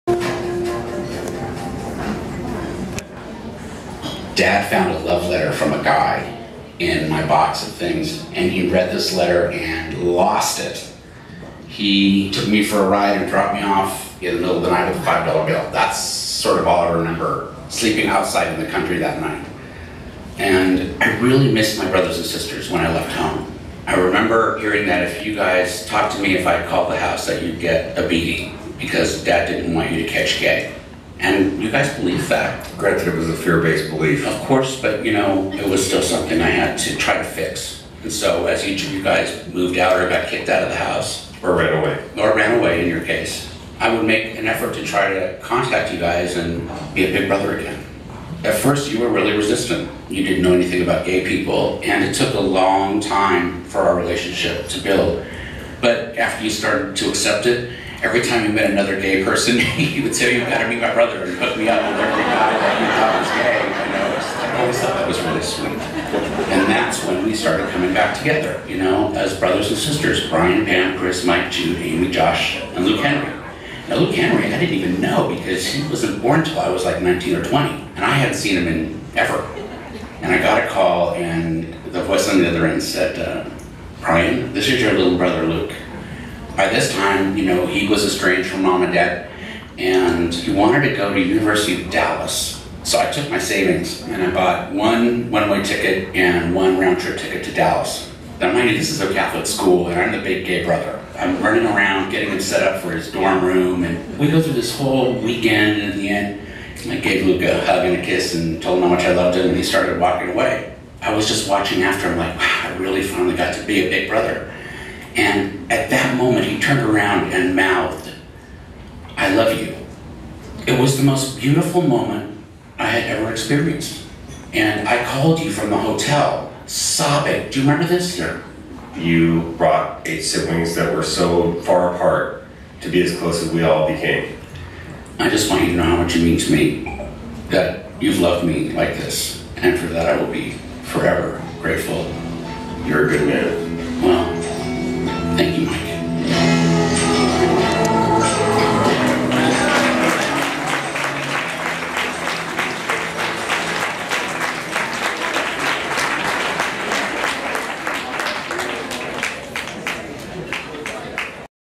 OUT LOUD GAY ANIMATION
An animation short produced by Story Corps to promote their new Out Loud project documenting untold diverse stories of LGBT peole across america.